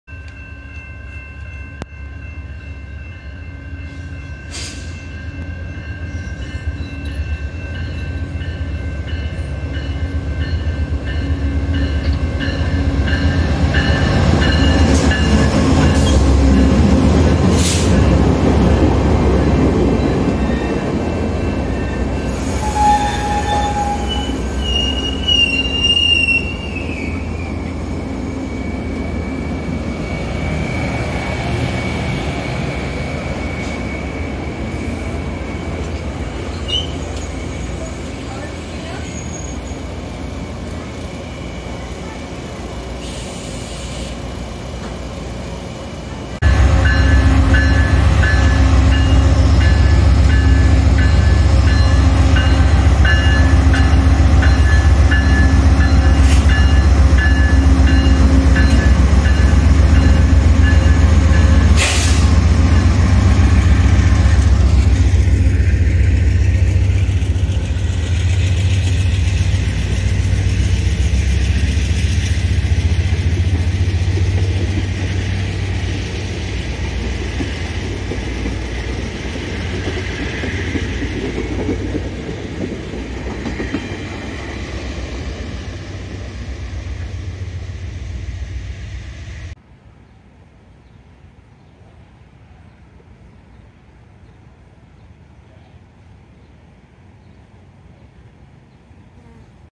VIA 73 at Chatham station. Jul 17, 2022
VIA 73 arriving and departing Chatham station.

VIA #901

CN Chatham Subdivision